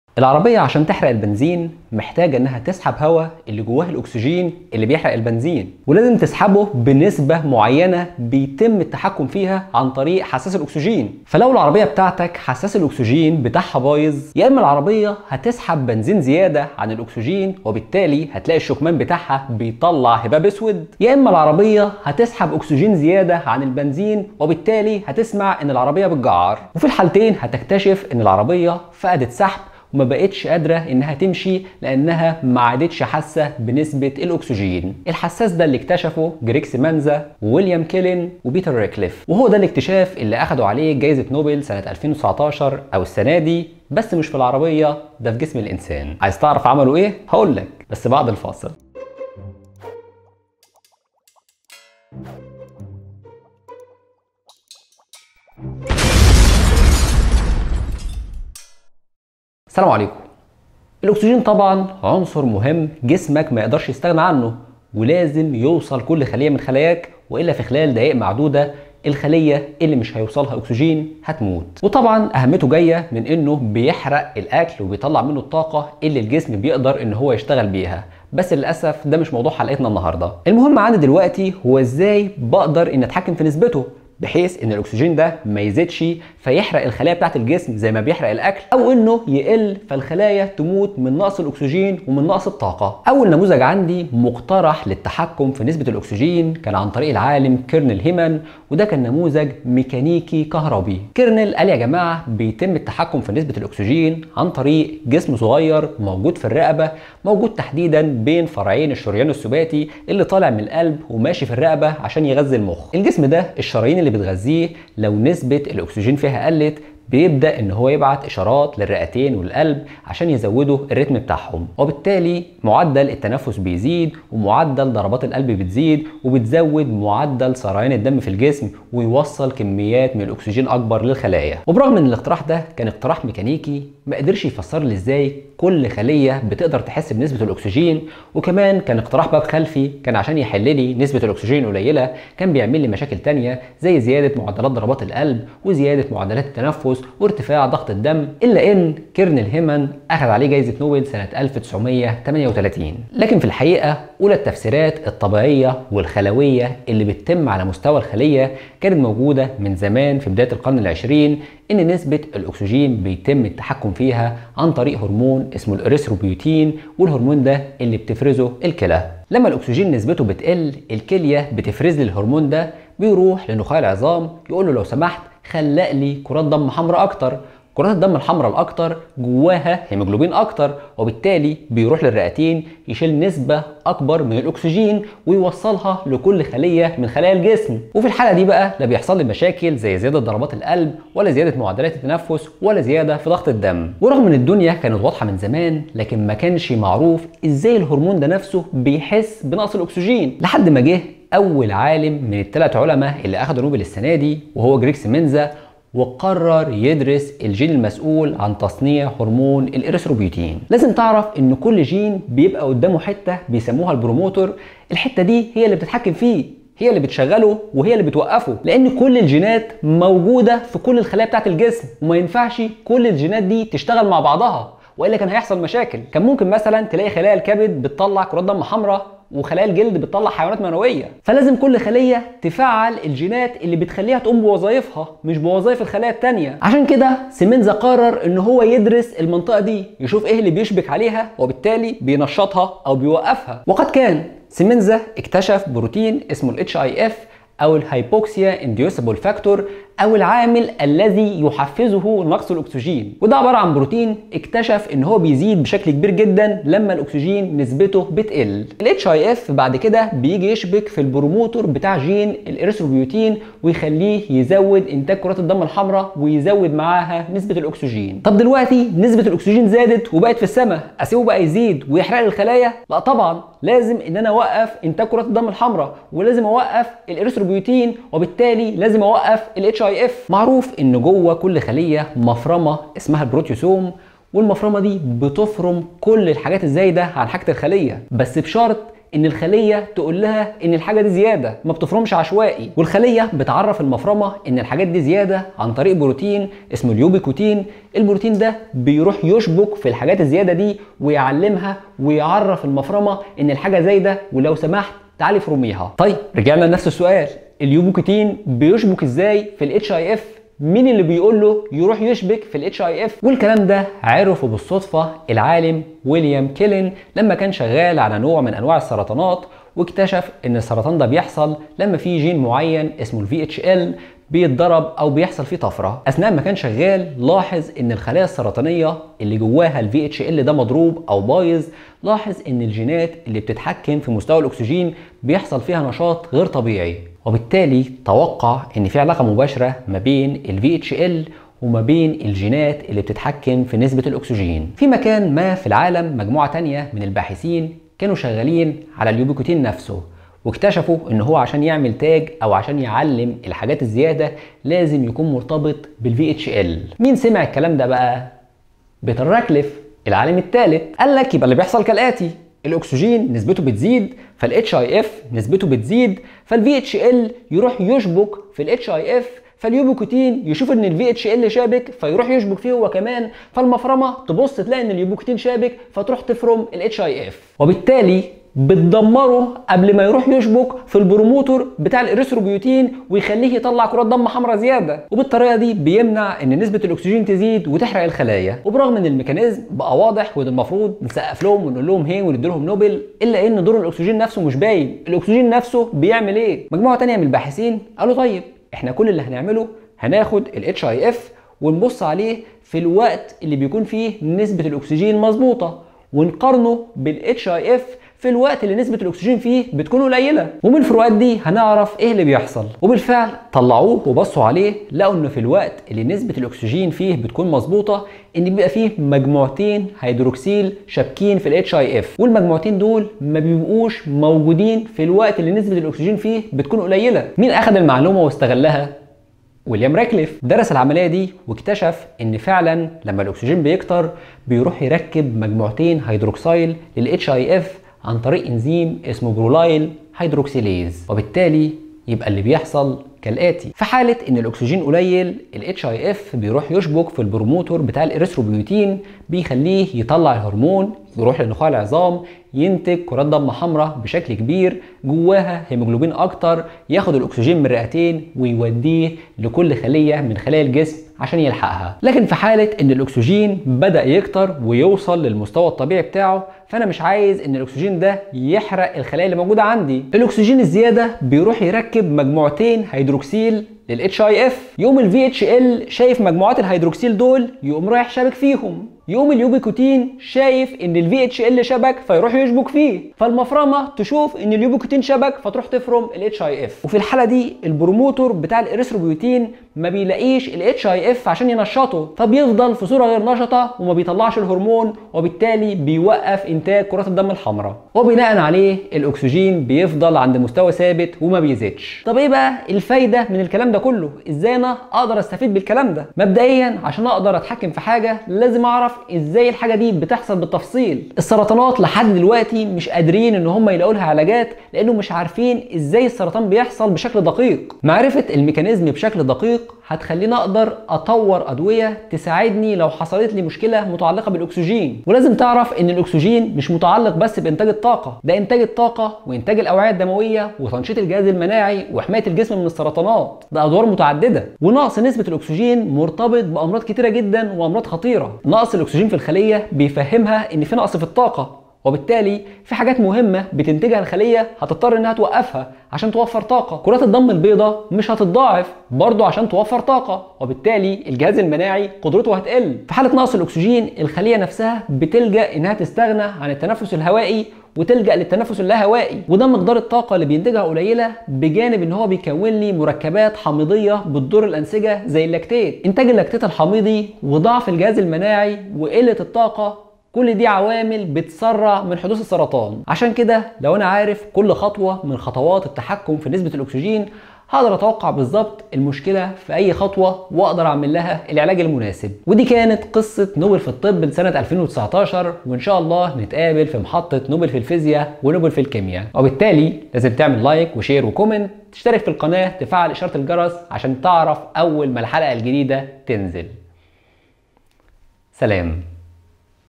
نوبل فى الطب 2019 | التحكم فى نسبة الاكسجين فى الجسم | حساس الاكسجين
حلقة استثنائيه متعلقه بجوايز نوبل 2019

نوبل ف الطب والفسيولوجيا 2019

كيف تستشعر الخليه وتتحكم بنسبة الاكسجين ؟
How cells sense and adapt to oxygen availability

William G. Kaelin, Gregg L. Semenza, Peter J. Ratcliffe